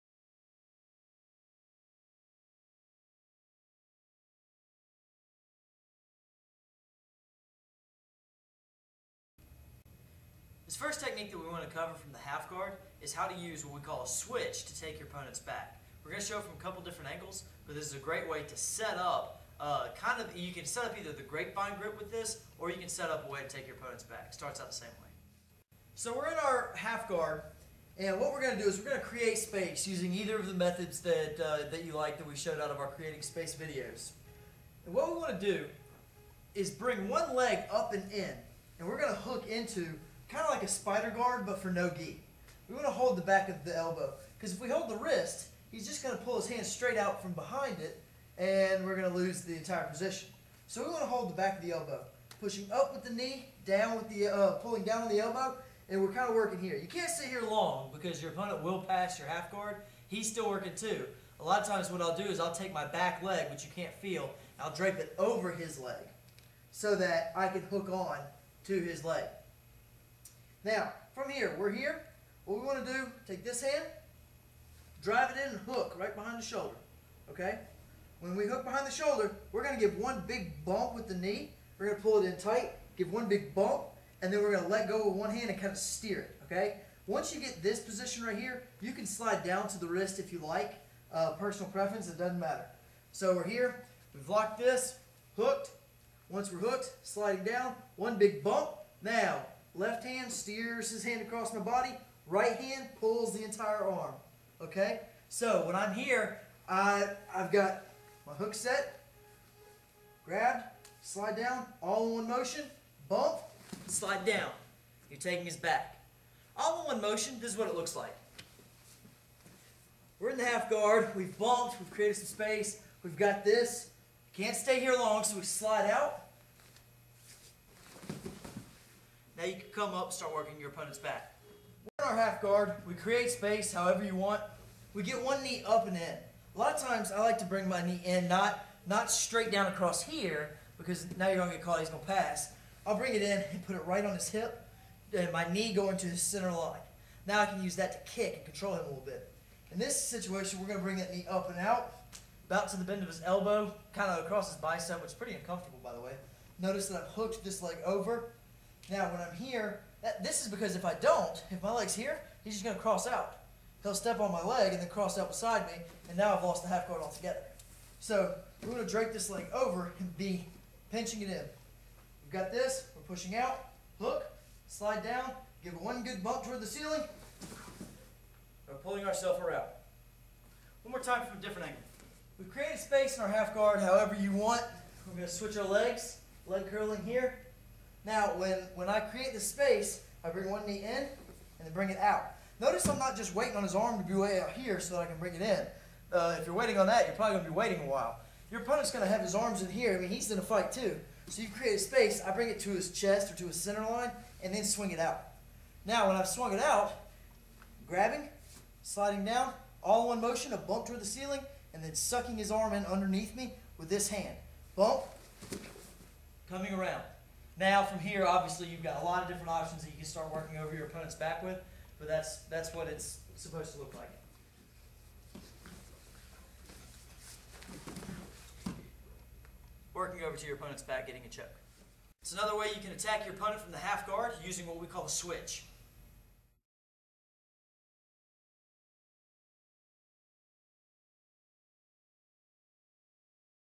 Arm Drag from Half-Guard "The Switch"
This is a modified arm drag from the half guard that we call "The Switch."

*Please note that we are fully aware that this is not "the switch" that is used in freestyle wrestling.*